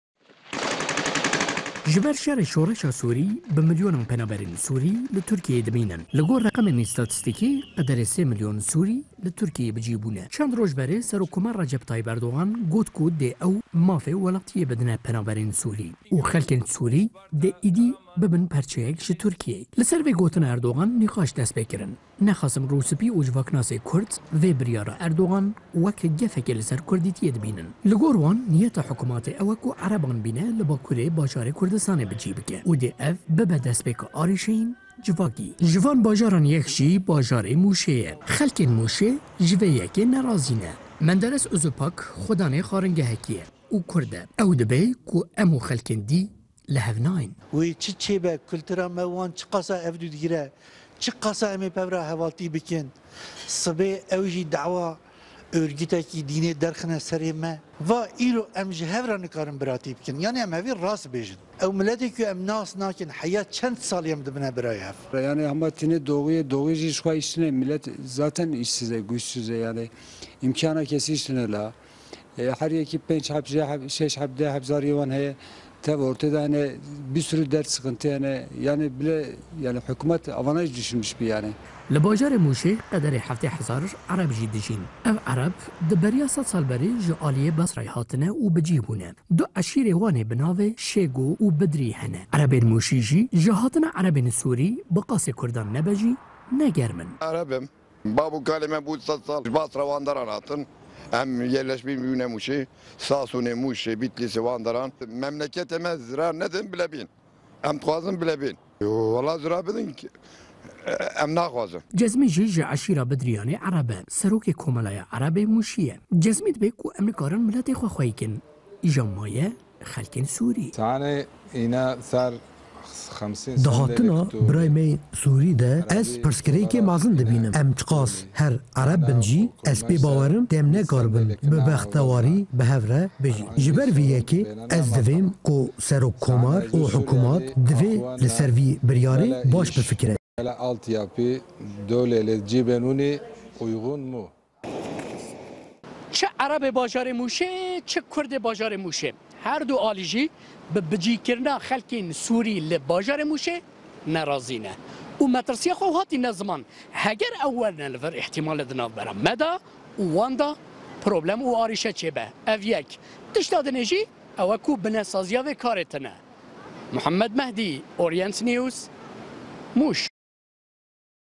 Xelkên bajarê Mûşê ji bicîkirina Erebên Sûrî nerazin e
Di beriya çend rojan ve Serokkomarê Tirkiyeyê Recep Teyîb Erdoğan amaje kir ku dê ew wek dewleta Tirkiye welatî bidine penaberên Sûrîyê, piştî çend rojan ev gengeşe gur bû, siyasetmedarên Kurd îddîa kirin ku penaberên Sûrî Erebin, û dê ew di bajarên bakûrê Kurdistanê de werine bi cîhkirin, yek ji wan navendan jî bajarê Mûşê ye, xwecihên Muşê ji vê yekê nerazîne û dixwazin ku hikumeta Tirk ji vî biryarê vegere